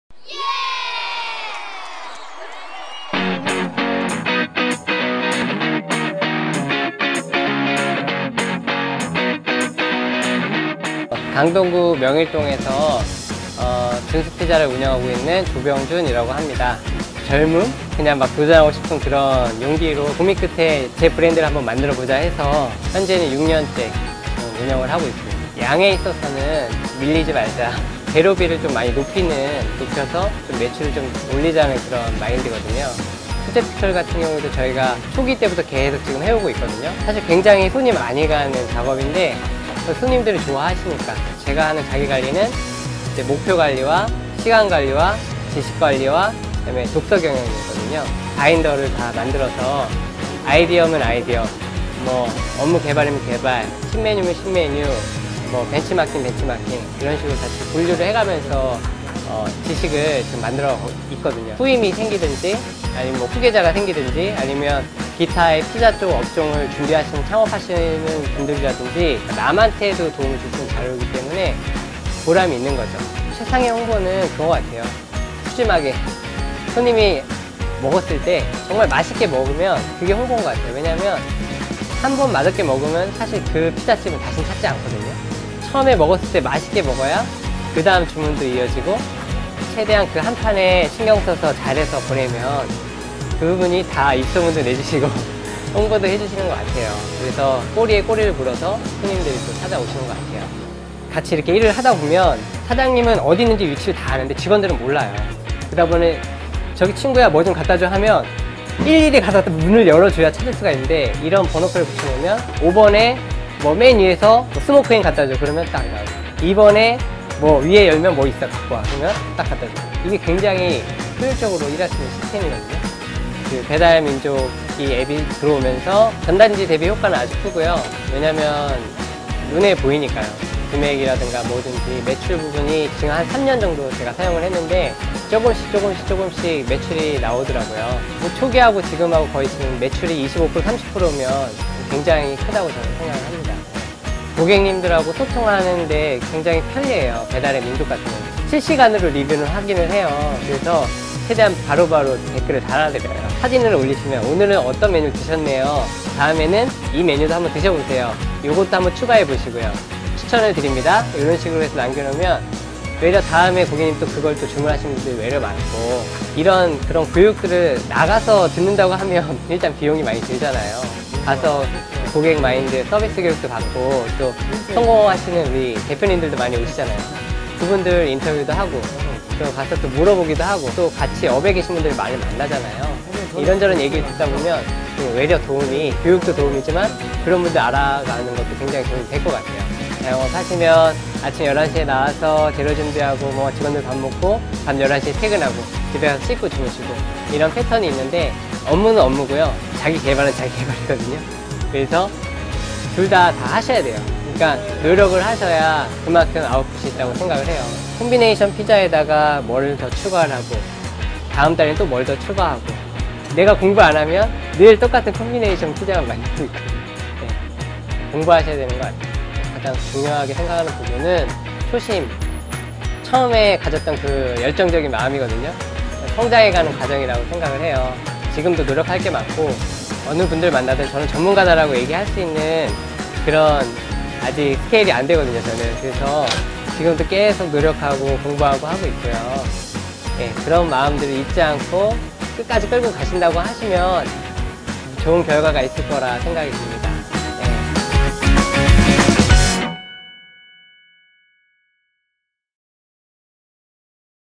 [배민아카데미] ‘푸짐함’이 최상의 홍보방법입니다
[배달의민족_성공업소 인터뷰] ‘푸짐함’이 최상의 홍보방법입니다.
2년간의 시행착오 끝에 당당하게 명일동 맛집으로 자리잡은 준스피자의 성공비법은?

배달의민족 성공업소 인터뷰에서 확인하세요!